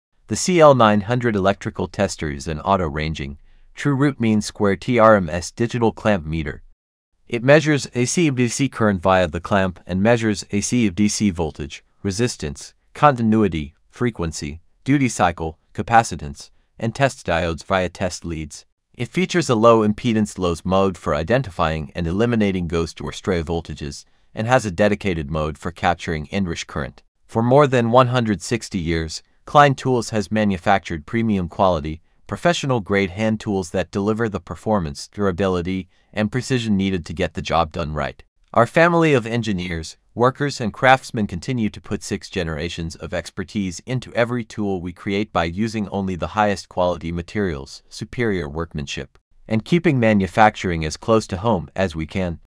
Klein Tools CL900 Digital Clamp Meter, Autoranging TRMS, AC/DC Voltage/Current
2000A, LoZ , Continuity, Frequency, NCVT, Temp, More, 1000V

Digital Clamp Meter measures 2000A AC/DC, 1000V AC/DC, 60 Milliohm.

Rugged Klein "TOUGH METER" industrial design stands up to jobsite demands; built to withstand a 6.6-Foot (2m) drop.

Automatically ranging True Root Mean Squared (TRMS) measurement technology for accuracy.

CAT IV 600V, CAT III 1000V, Class 2, double insulation safety rating
Backlit display for clear visibility in environments with low ambient lighting .